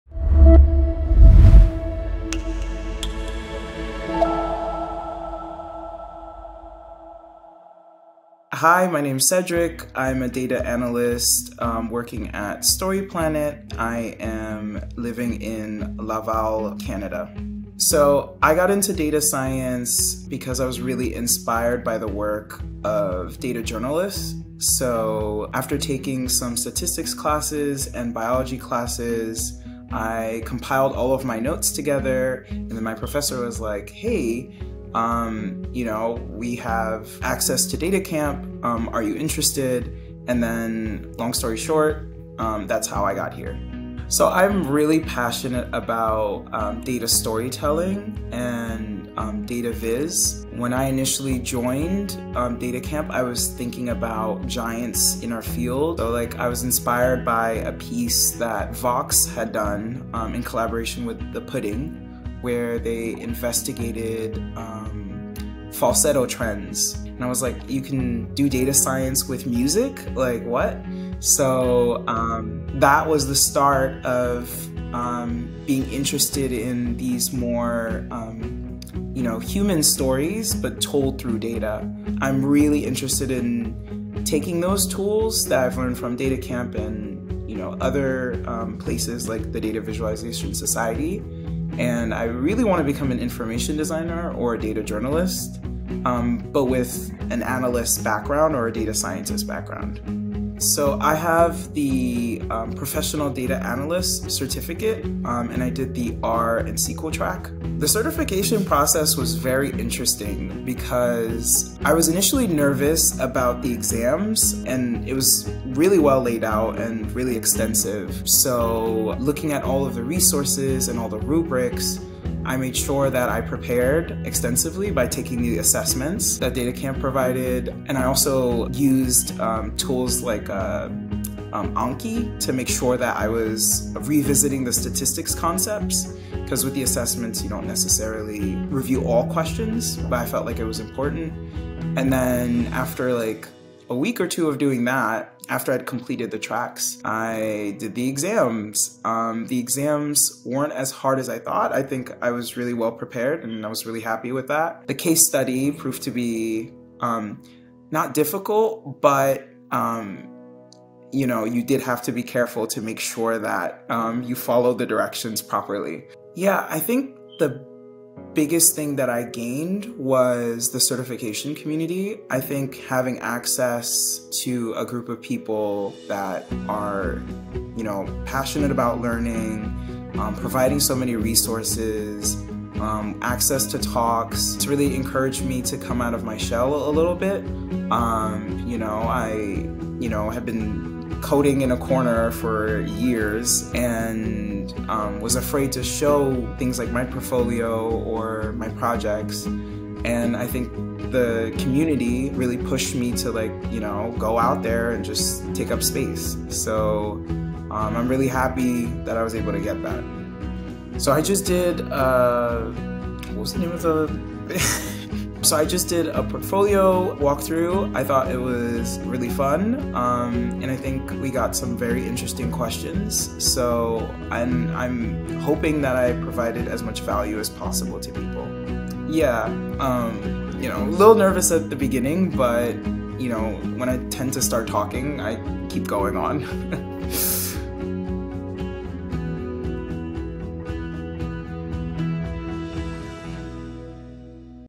Certified Learner Spotlight: Cedric Lary
One of our DataCamp Certified learners talks about his passion for data science, how he got into the field, and his experience with the DataCamp Certified Community.

Cedric is based in Canada and is working in the education industry while looking to pivot his career towards data science and information design. He recently did a live walkthrough and demo of his amazing portfolio for the members of our Certified Community.
You can connect with Cedric either via his portfolio site or via his LinkedIn if you'd like to know more!